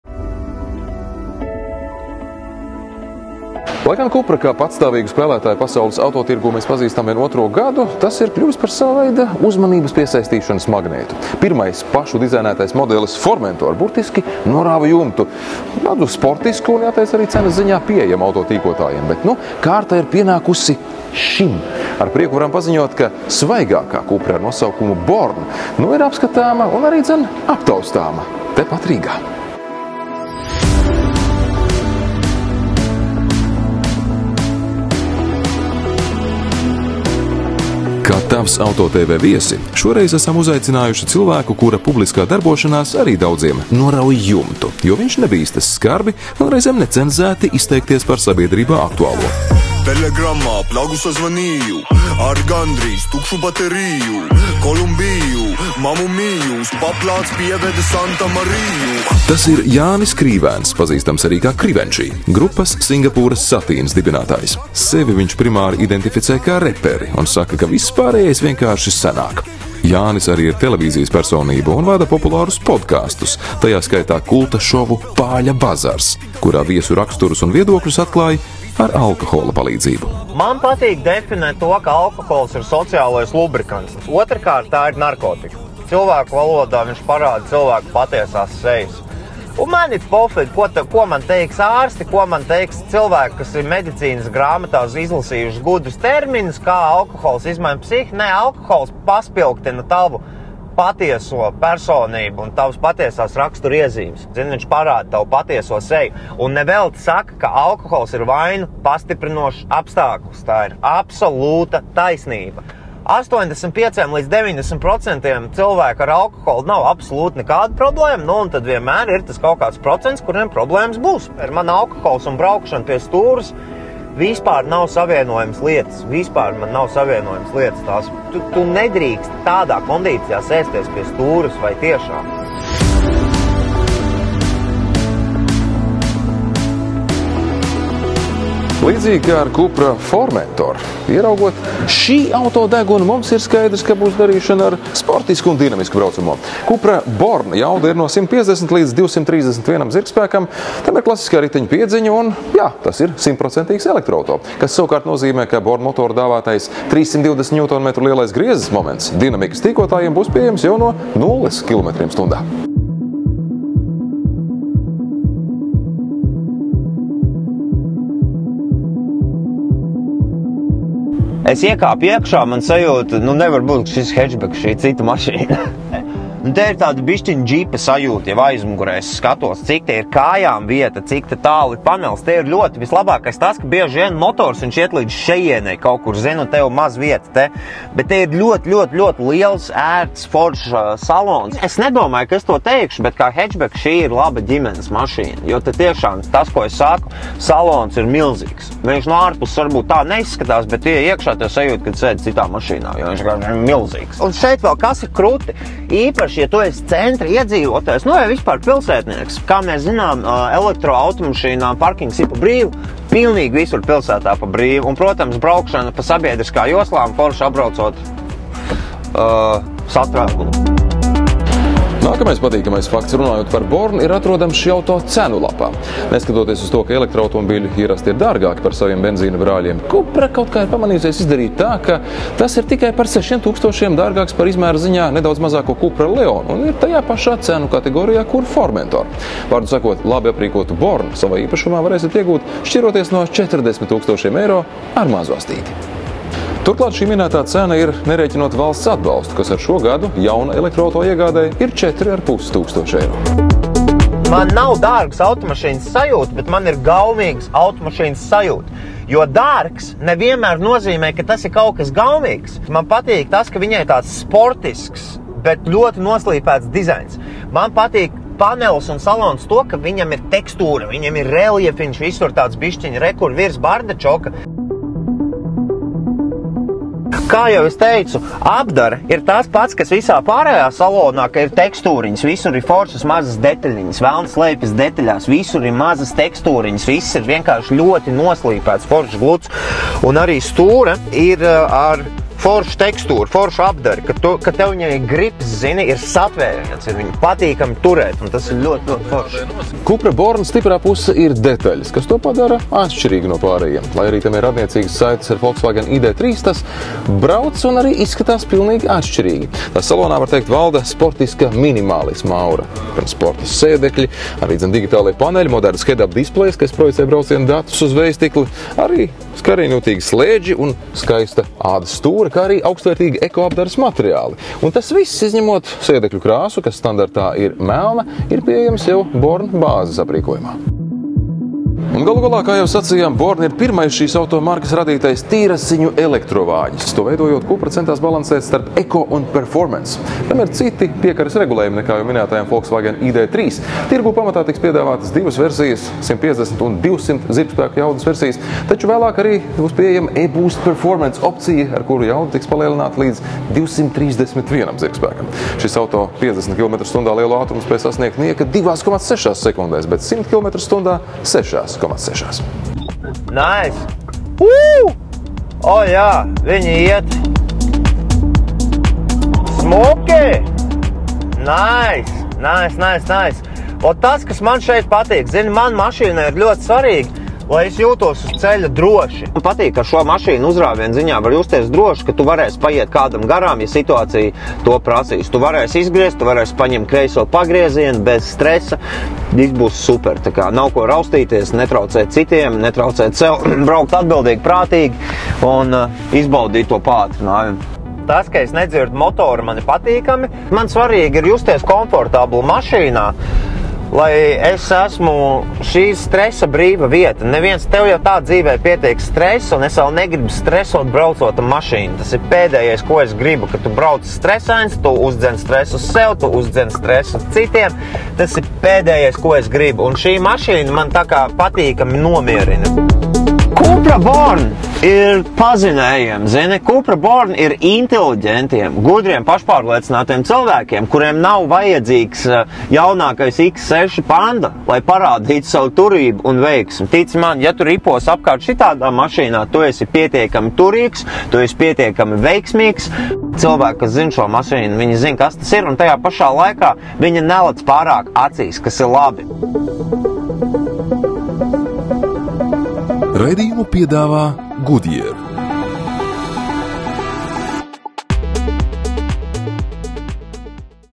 CUPRA BORN PIRMAIS TESTA BRAUCIENS LATVIJĀ!
Lai gan zīmolu Cupra kā patstāvīgu spēlētāju pasaules autotirgū pazīstam vien otro gadu, tas jau ir paguvis kļūt par tādu kā uzmanības piesaistīšanas magnētu. Pirmais pašu patstāvīgi dizainētais modelis Formentor burtiski norāva jumtu daudziem sportisku un, galvenais, cenā pieejamu auto tīkotājiem. Bet tagad kārta ir pienākusi šim! Ar prieku varam paziņot, ka svaigākā CUPRA ar nosaukumu BORN jau šobrīd ir apskatāma un arīdzan aptaustāma Rīgā!

Kā Tavs Auto TV viesi šoreiz esam uzaicinājuši cilvēku, kura publiskā darbošanās arī daudziem „norauj jumtu”, jo viņš nebīstas skarbi (un reizēm necenzēti) izteikties par sabiedrībā aktuālo. Tas ir Jānis Krīvēns, pazīstams arī kā Krivenchy - grupas „Singapūras Satīns” dibinātājs. Sevi viņš primāri identificē kā "reperi" un saka, ka "viss pārējais vienkārši sanāk". Jānis arī ir televīzijas personība un vada populārus podkāstus, t.sk. kulta šovu "PĀĻA BAZARS", kurā viesu raksturus un viedokļus atklāj ar alkohola palīdzību.